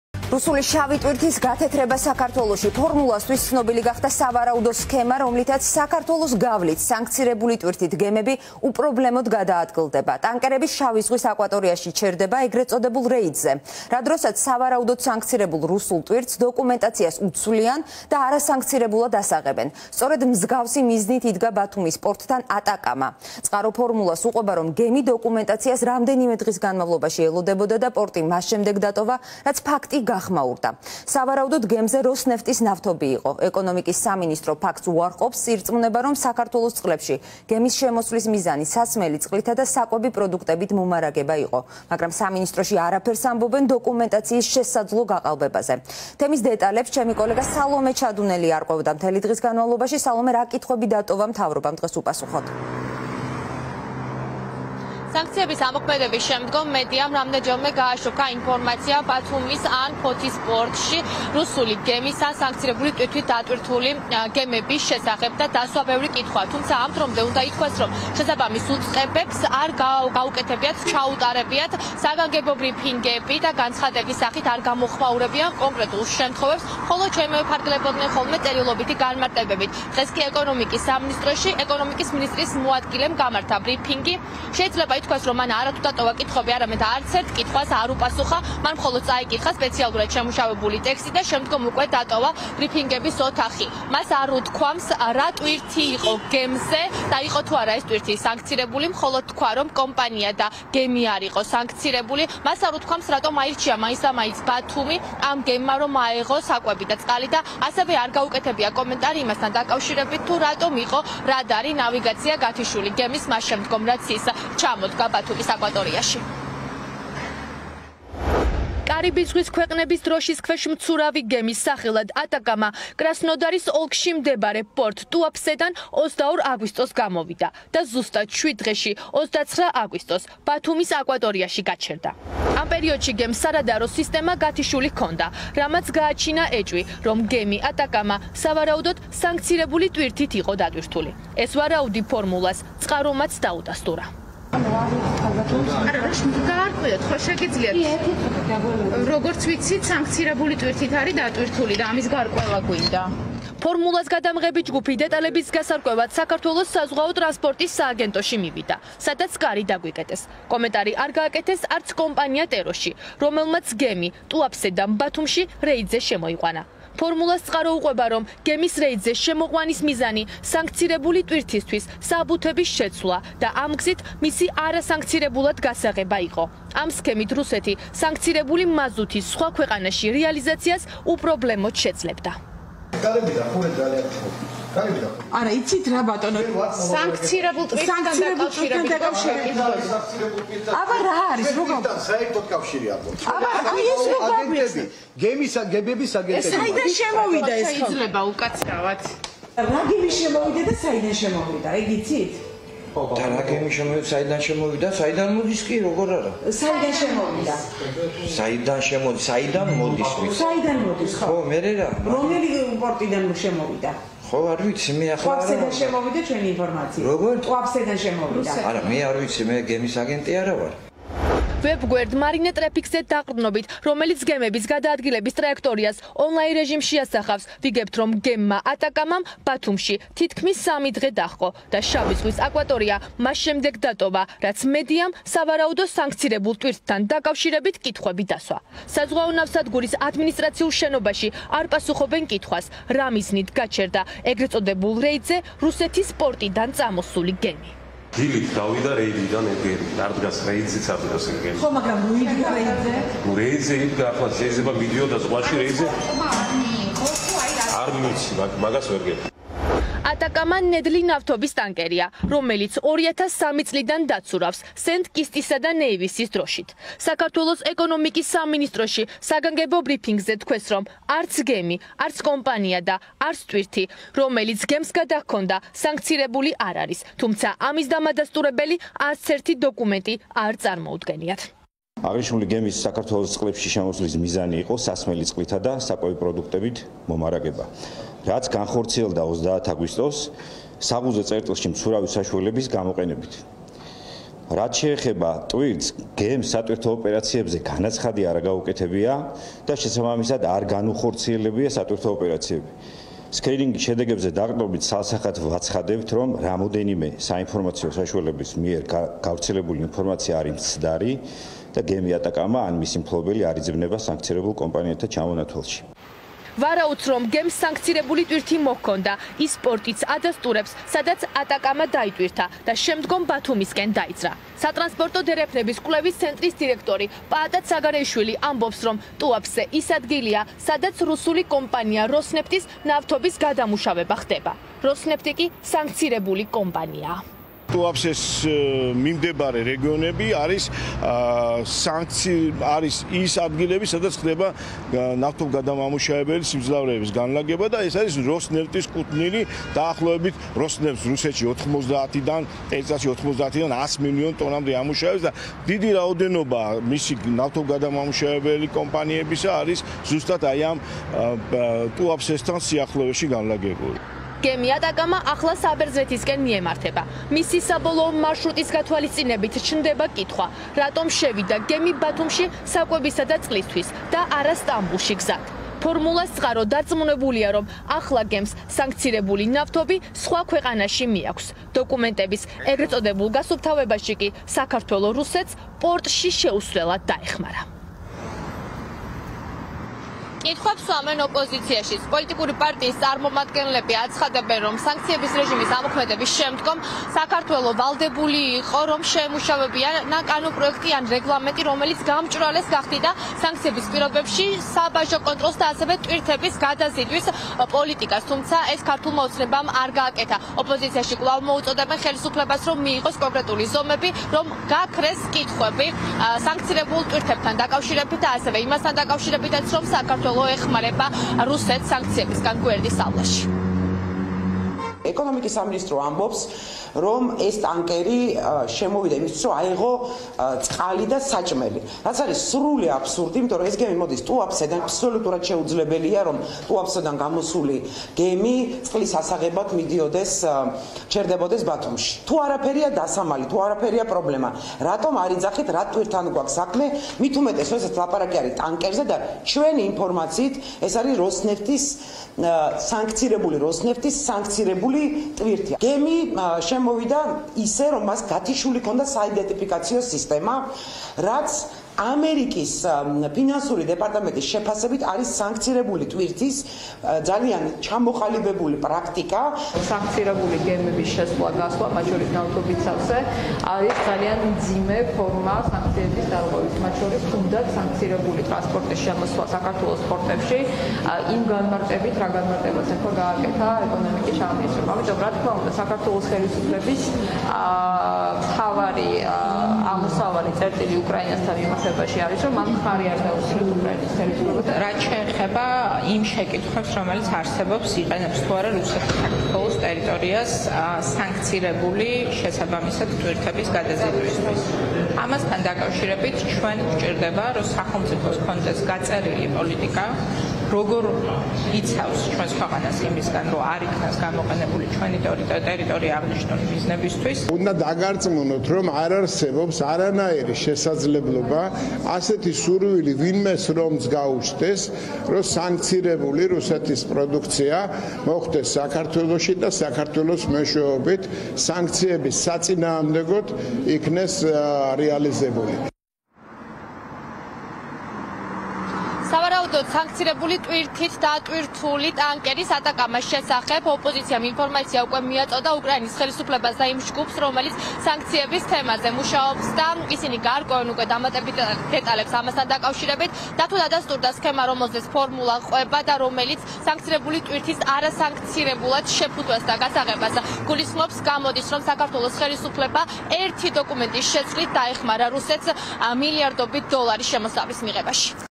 გემი სანქცირებული ტვირთით
▪️ რუსული შავი ტვირთის გათეთრება საქართველოში — ფორმულამ გაარკვია დეტალები სავარაუდო სქემაზე, რომლითაც საქართველოს გავლით გემები სანქცირებული ტვირთით უპრობლემოდ გადაადგილდება